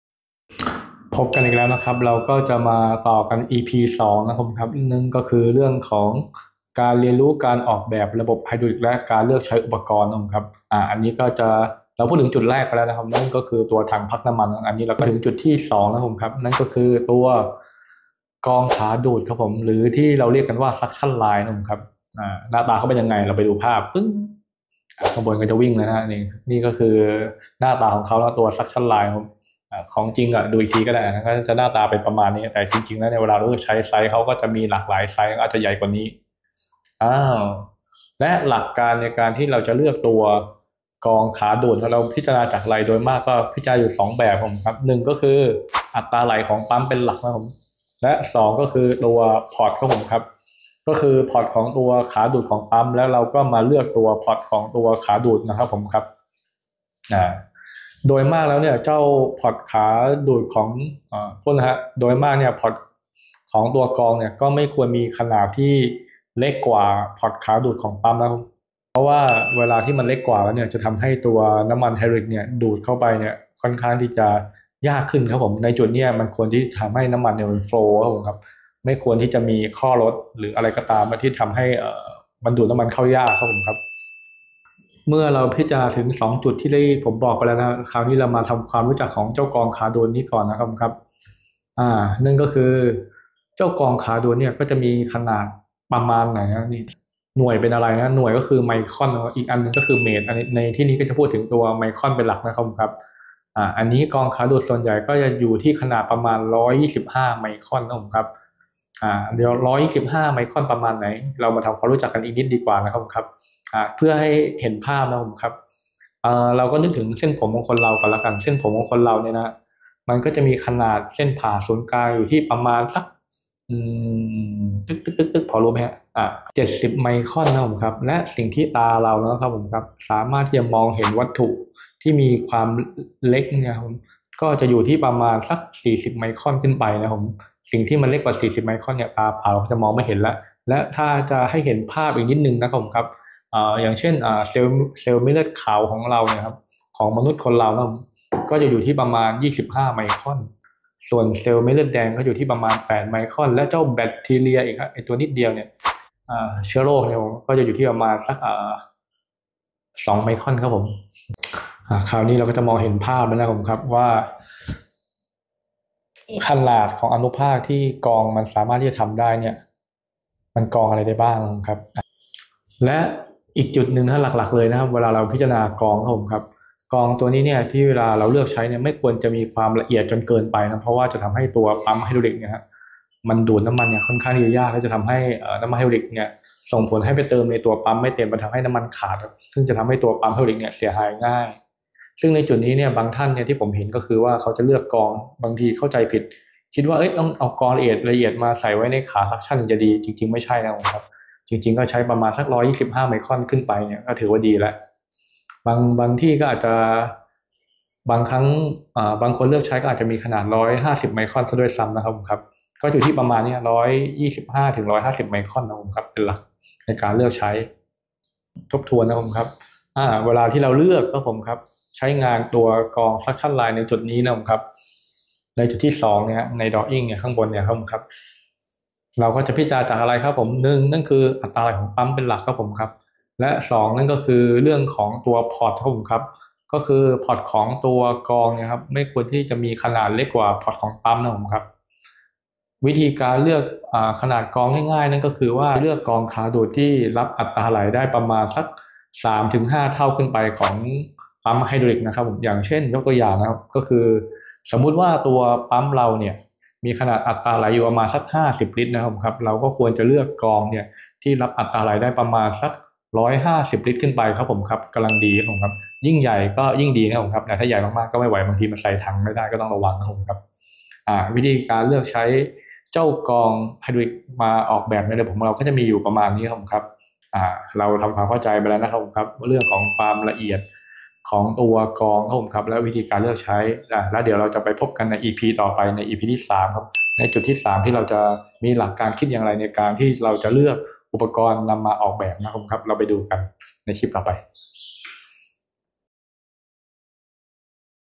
เรียนรู้การออกแบบระบบ และเลือกอุปกรณ์ไฮดรอลิค แบบง่าย ๆ(ออนไลน์) EP.2
เรียนรู้การออกแบบระบบ และ
เลือกอุปกรณ์ไฮดรอลิค แบบง่าย ๆ(ออนไลน์) สำหรับบุคลทั่วไป นำไปใช้ง่าย ๆ เบื้องต้น( มาต่อ Ep 2 กันเลย)
EP.2 การเลือก กรองขาดูด (Strainer) Suction line มาใส่ในระบบไฮดรอลิค ที่ต้องการออกแบบ.